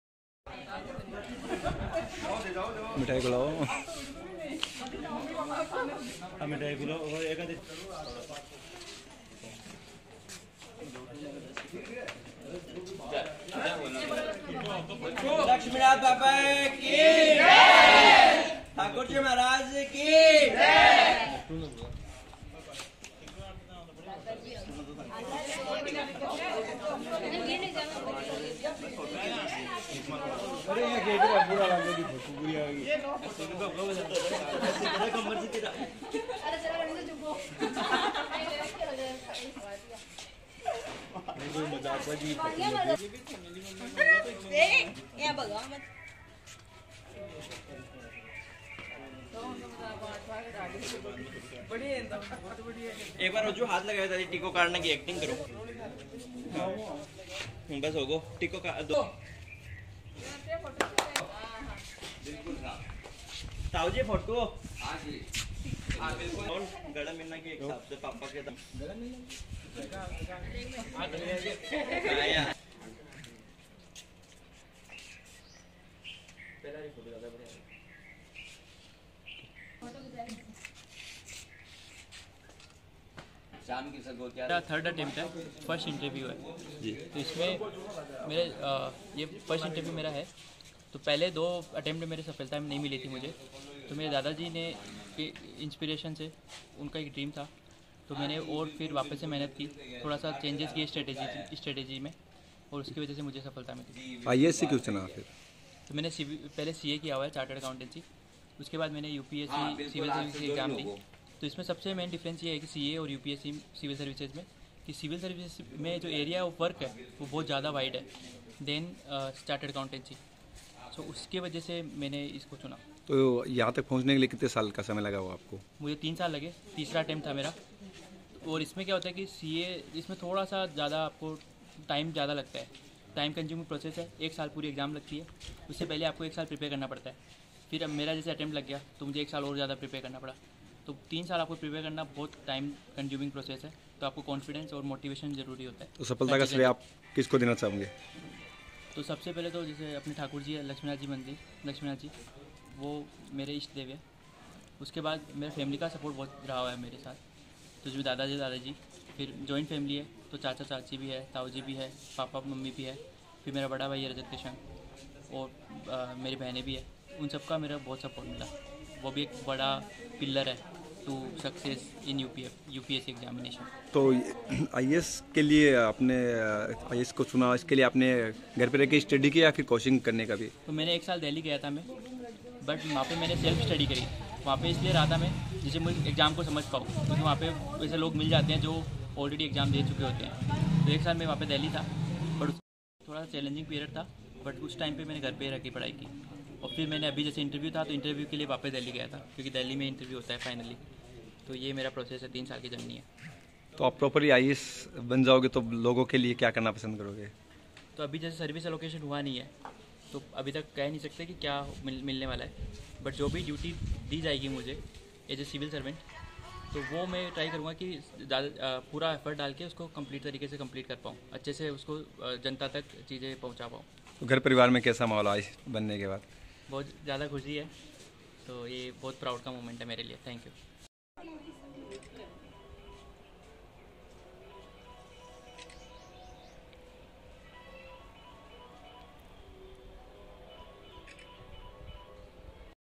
फतेहपुर के 25 वर्षीय युवा रोहन केसान का यूपीएससी आईएएस में 387 रैंक के साथ हुआ चयन
IAS